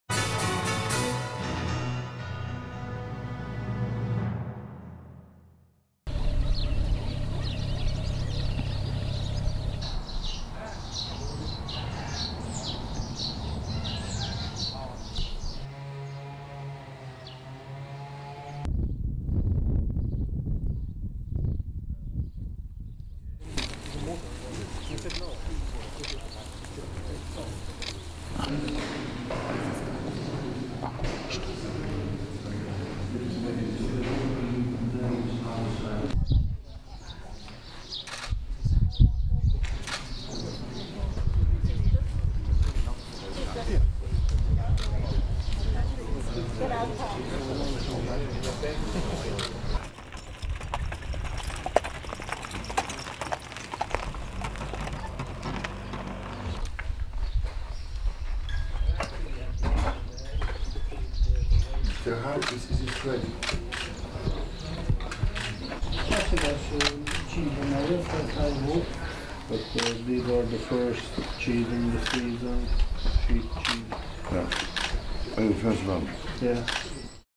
Prinţul Charles a vizitat satul Viscri şi 3 biserici medievale din judeţul Braşov
Prinţul Charles a revenit în România într-o vizită privată, de această dată, ocazie cu care a mers în satul Viscri, unde a discutat cu producătorii locali, şi a vizitat 3 biserici medievale, construite în jurul anilor 1200, în satele Drăușeni, Homorod și Mercheasa, din judeţul Braşov.